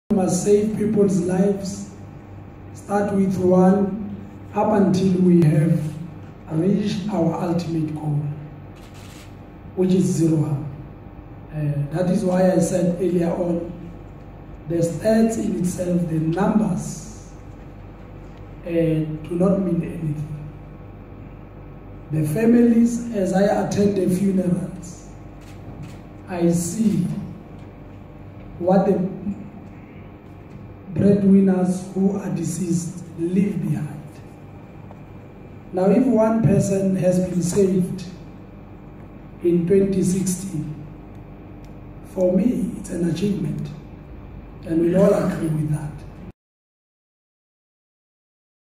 More work needed to ensure mineworker safety: Zwane
Mineral Resources Minister Mosebenzi Zwane says 73 mineworkers died in 2016.